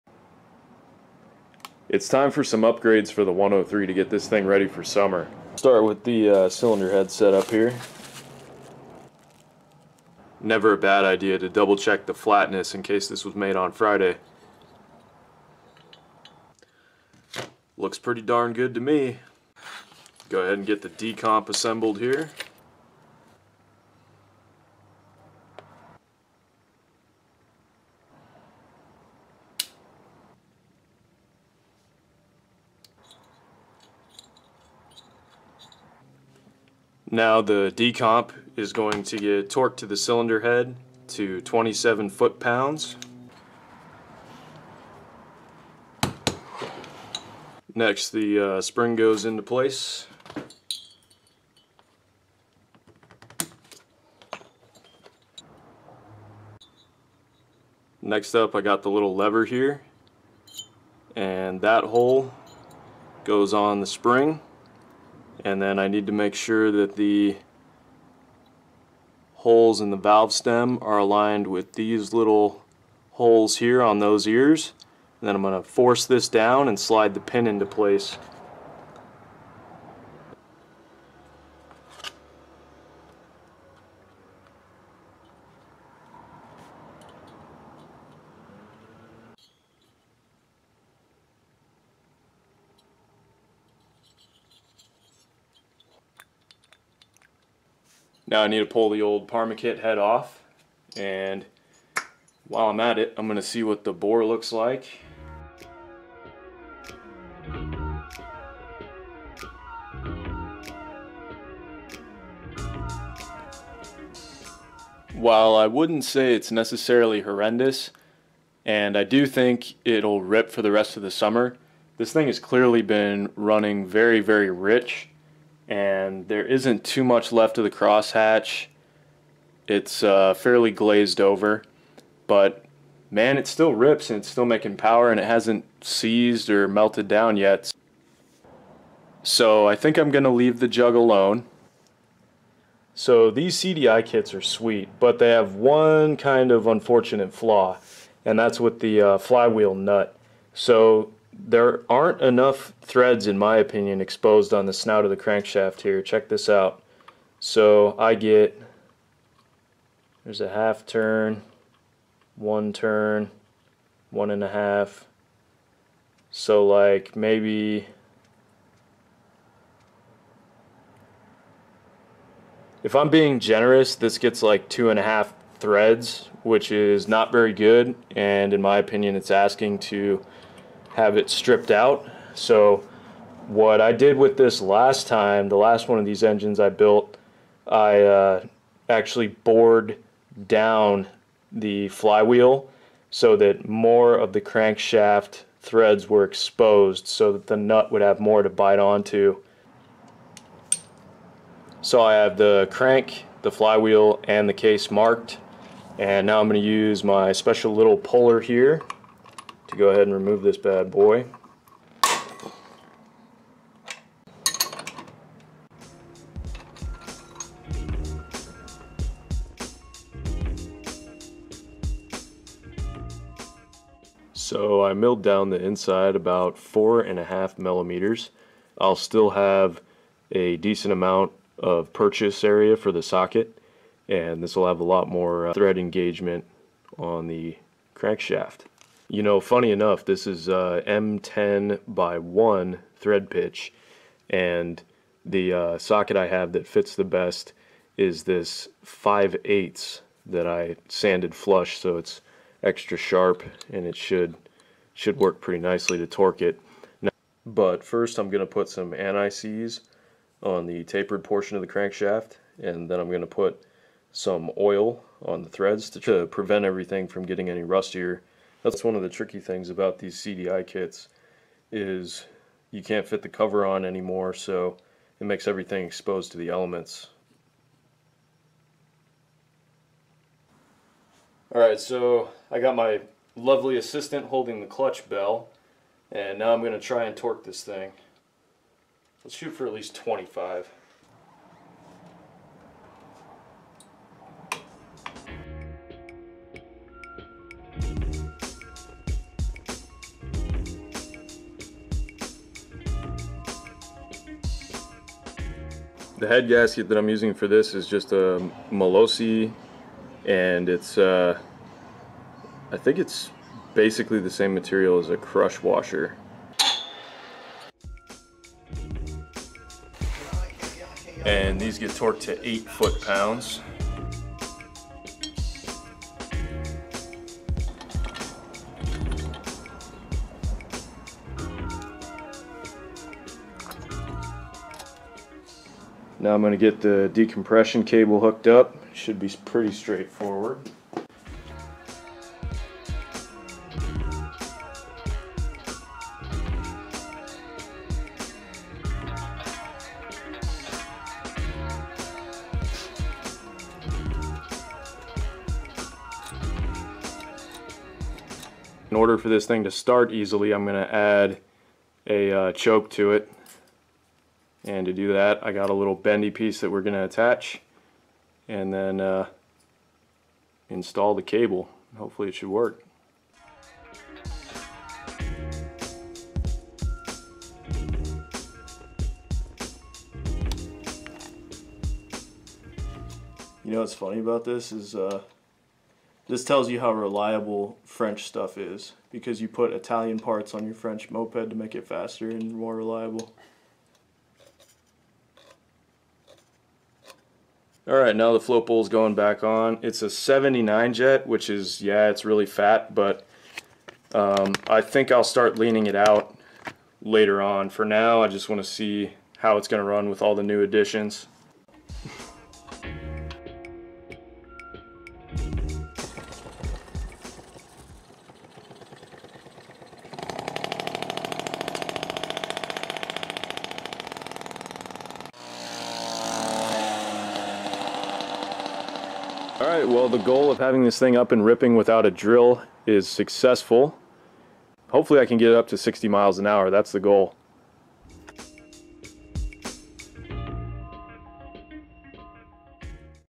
Peugeot 103 70cc, cylinder head, decompression valve, CDI, Timing, Dellorto
I install a Polini 70cc decomp head to match the jug on my 103. I build up and install a new decompression valve for it, and get that installed along with a cable so that it's easy to start! I used a full metal headgasket. I pull off the flywheel to show an important modification if you run the Treatland Le Partie CDI kit, machining it so that the crank won't strip when it's torqued. I get the timing set and fix the choke on the Dellorto carburetor as well! I included torque specs also.